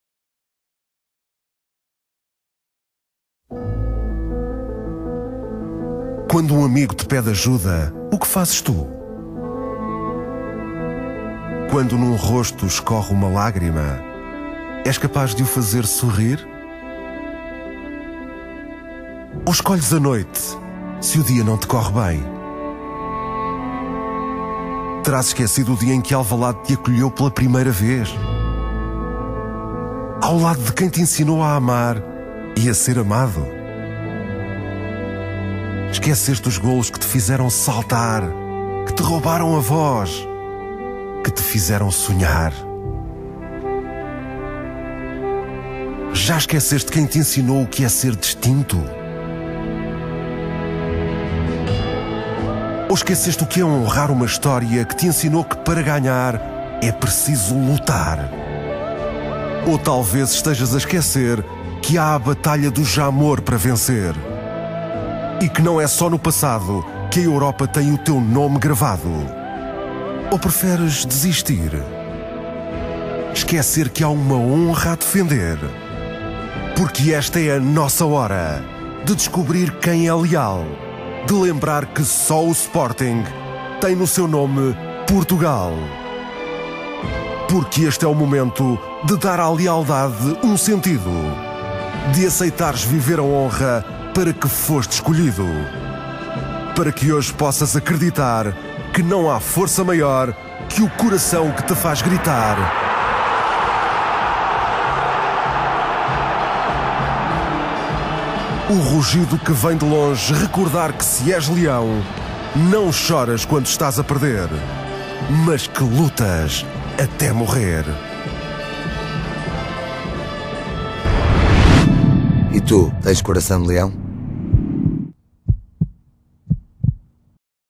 Nós acreditamos no Sporting. E tu?
Este é o momento! São nos momentos mais difíceis que se encontram os adeptos mais leais. Este é o momento para que todos fomos escolhidos. O momento para lutar até ao fim. Acreditar no Sá Pinto e na equipa.
Nós acreditamos. E tu?